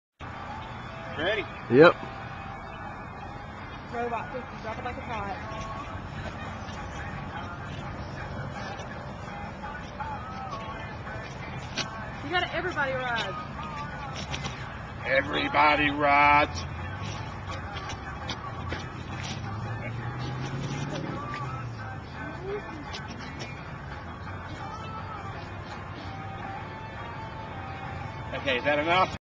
Mr. Whitlock Does The WORST Robot Ever @ Frank Myers Auto
- Uncle Frank was looking through an old PC and found some videos from a contest held several years ago. The contest was "Dancing With The Cars", held at Frank Myers Auto Maxx in Winston Salem, and the video footage was thought to be lost...UNTIL NOW! Here is a portion of one of those infamous contest videos.